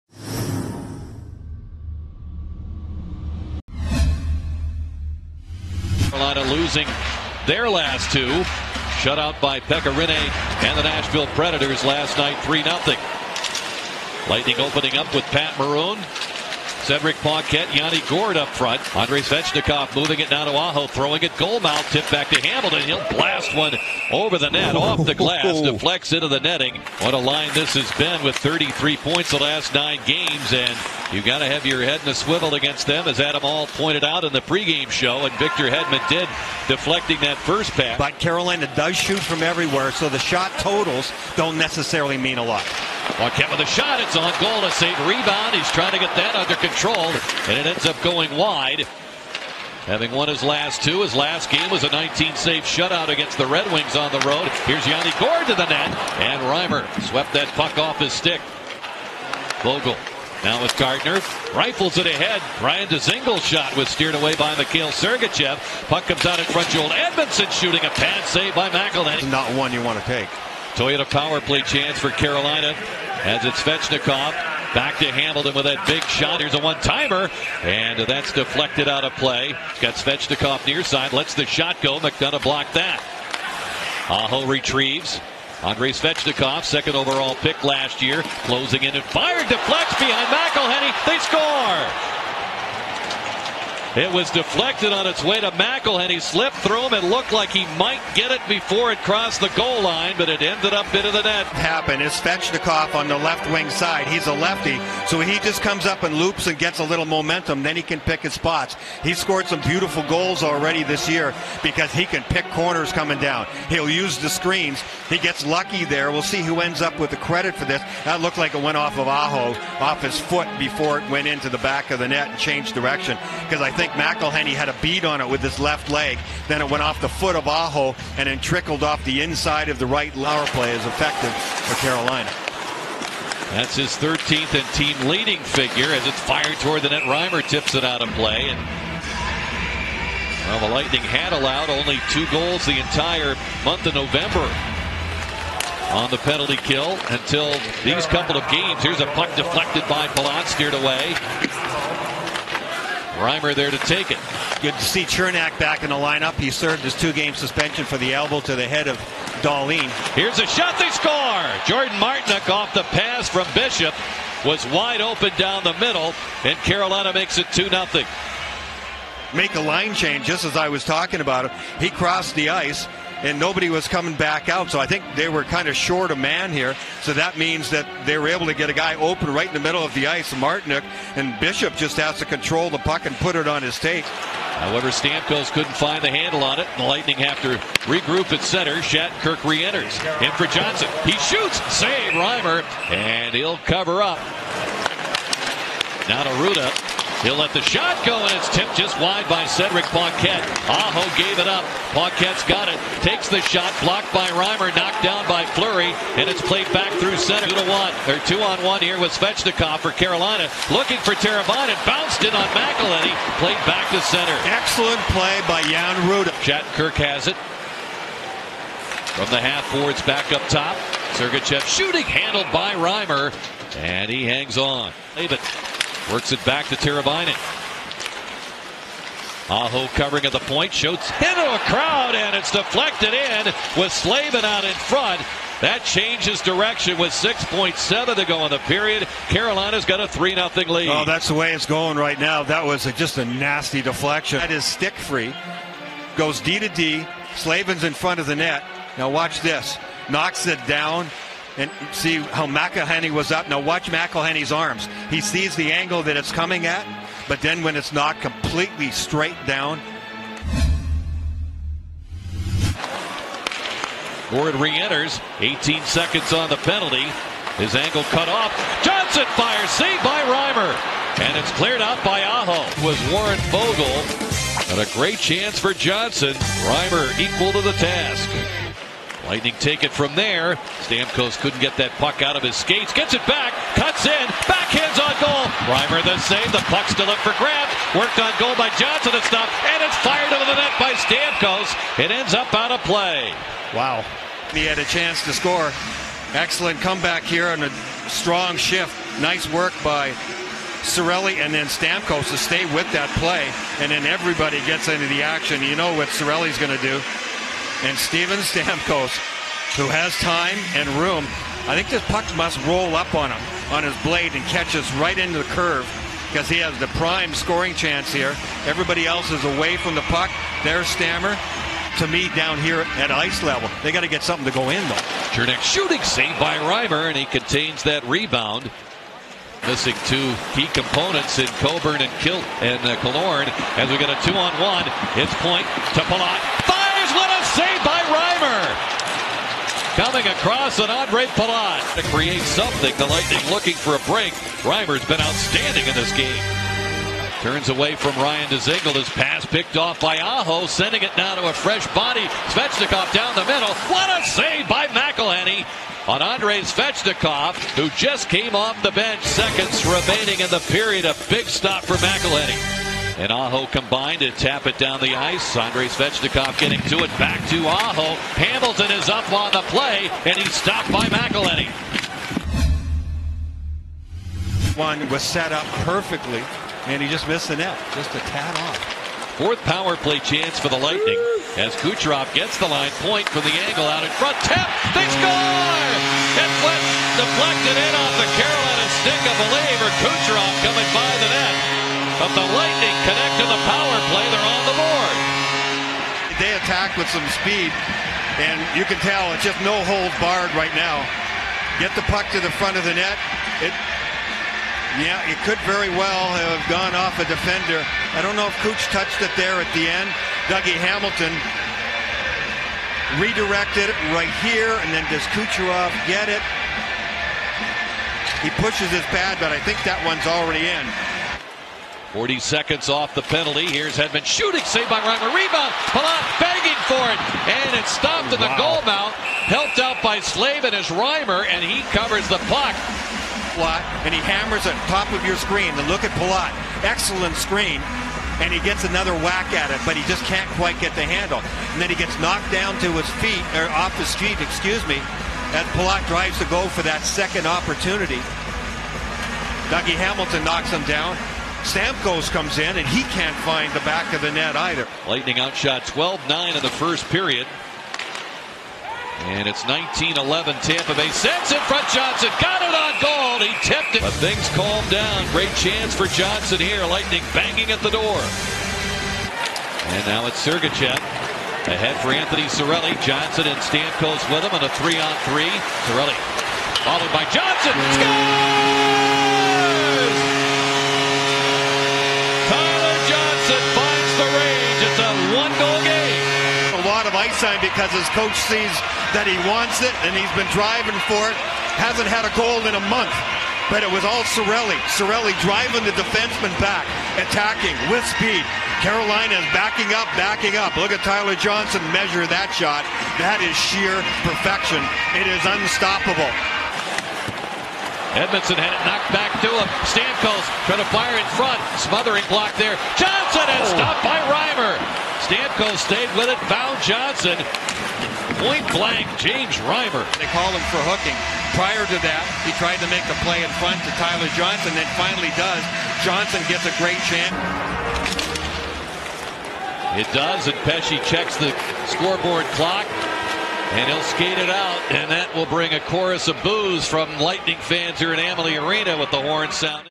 NHL Highlights | Hurricanes @ Lightning 11/30/19
Extended highlights of the Carolina Hurricanes at the Tampa Bay Lightning